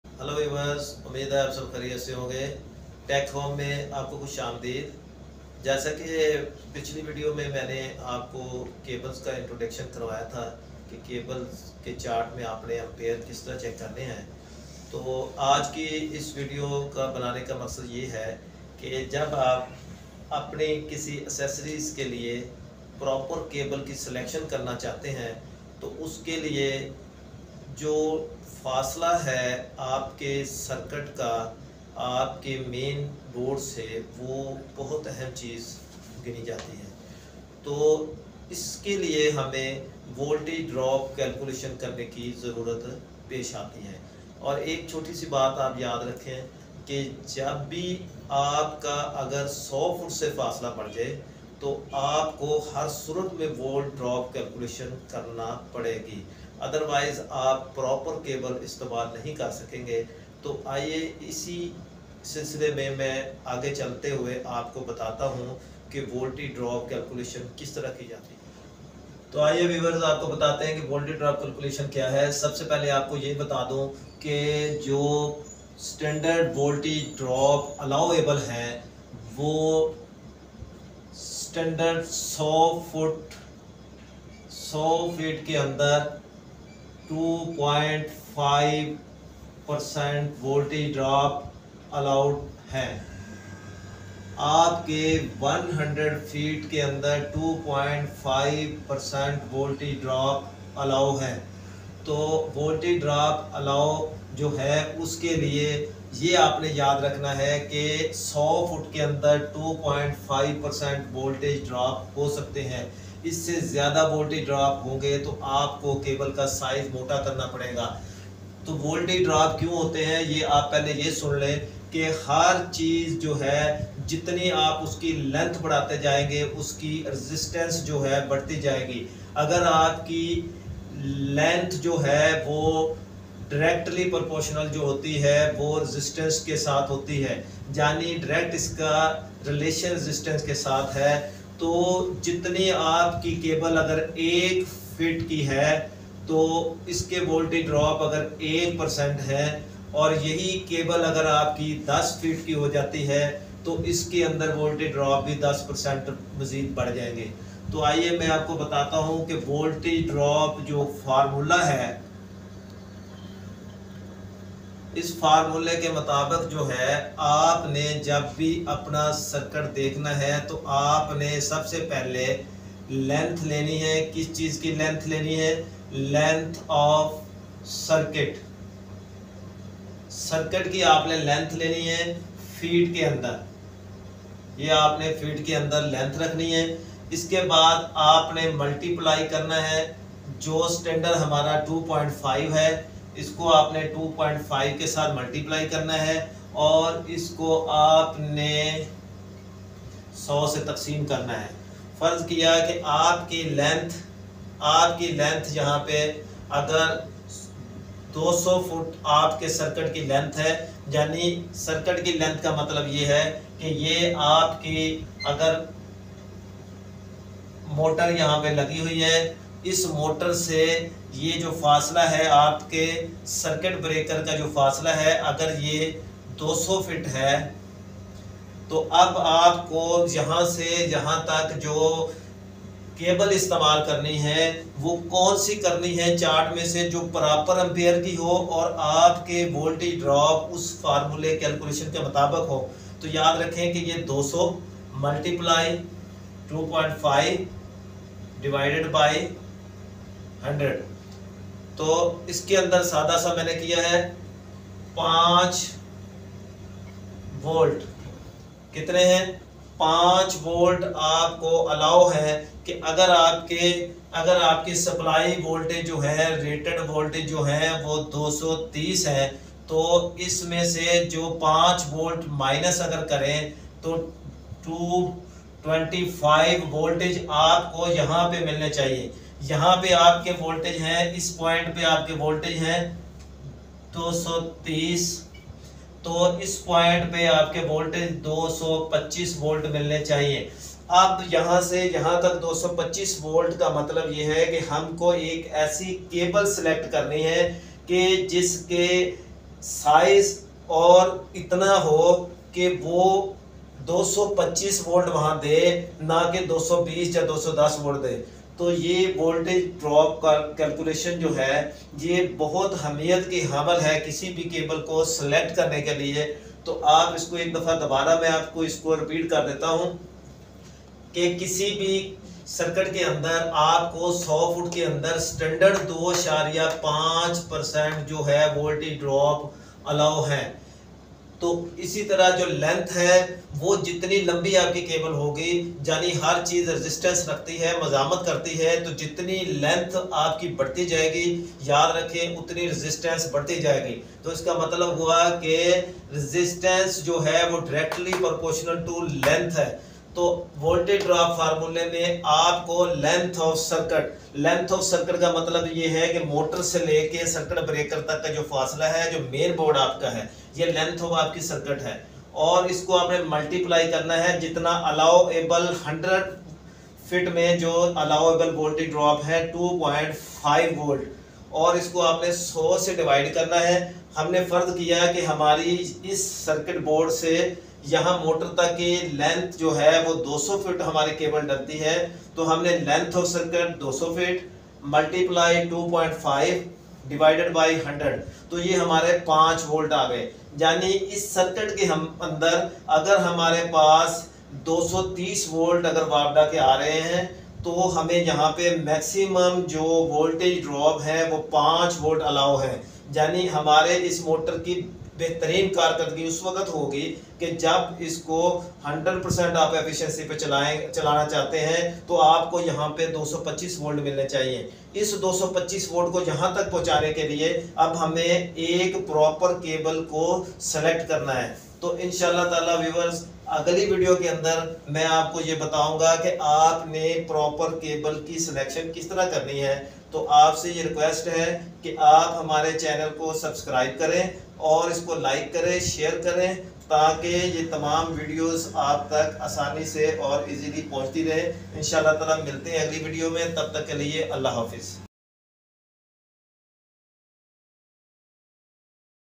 How to calculate voltage drop in electric circuit @TechHome1995 nayyar abbas
I have try to explain in this video,how to calculate voltage drop in electric circuit for good efficiency of electric appliances@TechHome1995